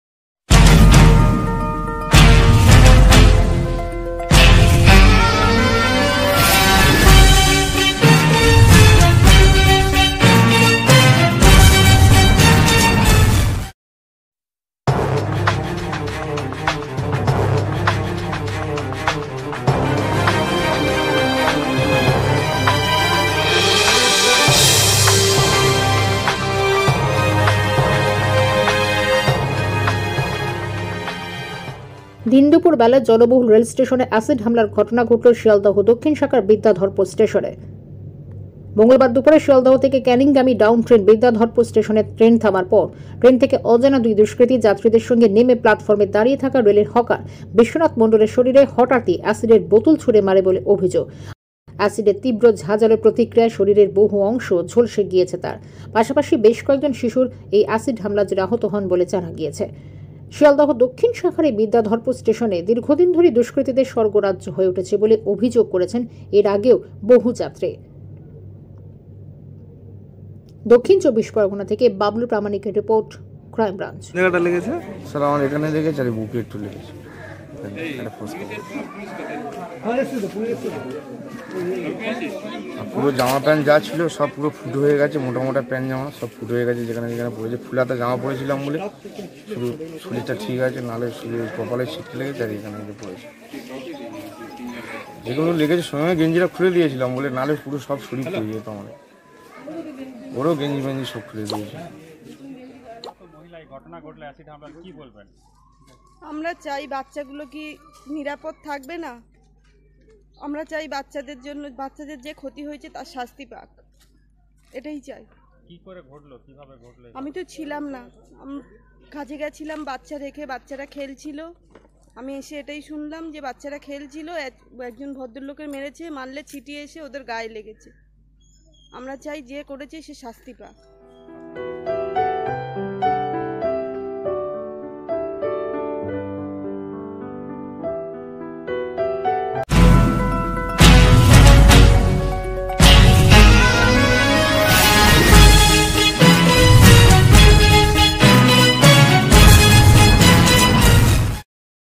শিয়ালদহ দক্ষিণ শাখার বিদ্যাধরপুর স্টেশনে অ্যাসিড হামলা, ঝলসে গেল রেল হকার
বাবলু প্রামানিক। দঃ২৪পরগনা। ক্রাইম ব্রাঞ্চ।

দিন দুপুরবেলায় জনবহুল রেল স্টেশনে অ্যাসিড হামলার ঘটনা ঘটলো শিয়ালদহ দক্ষিণ শাখার বিদ্যাধরপুর স্টেশনে। গতকাল দুপুরে শিয়ালদহ থেকে ক্যানিং গামী ডাউন ট্রেন বিদ্যাধরপুর স্টেশনে থামার পরে ট্রেন থেকে অজানা  দুই দুষ্কৃতী যাত্রীদের সঙ্গে নেমে প্লাটফর্মে দাঁড়িয়ে থাকা রেলের হকার বিশ্বনাথ মন্ডলের শরীরে অ্যাসিডের বোতল ছুঁড়ে মারে। তীব্র ঝাঁঝালো অ্যাক্সিডেন্ট প্রতিক্রিয়া শরীরের বহু অংশ ঝলসে যায় । পাশাপাশি  বেশ কয়েকজন শিশুর অ্যাসিড হামলার জেরে আহত হয়েছে বলে জানা গিয়েছে।
প্রসঙ্গত শিয়ালদহ দক্ষিণ শাখার এই বিদ্যাধরপুর স্টেশন এ দীর্ঘদিন ধরেই দুষ্কৃতীদের স্বর্গরাজ্য হয়ে উঠেছে বলে এর আগেও বহুবার শিয়ালদহ জিআরপি এবং নিকটস্থ সোনারপুর থানায় লিখিত অভিযোগ দায়ের করেছে স্থানীয় প্রশাসন।